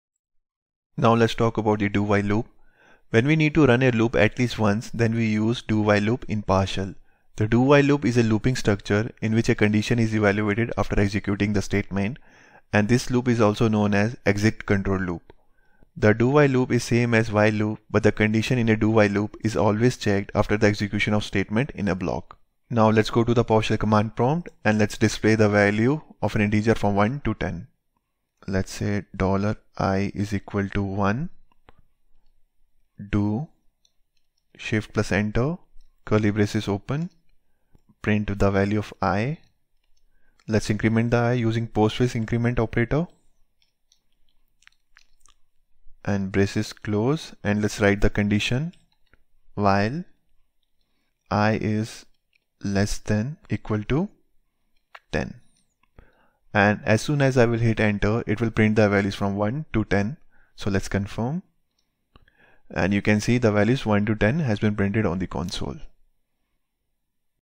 PowerShell: DO WHILE LOOP
In this video we will learn about Do While loop.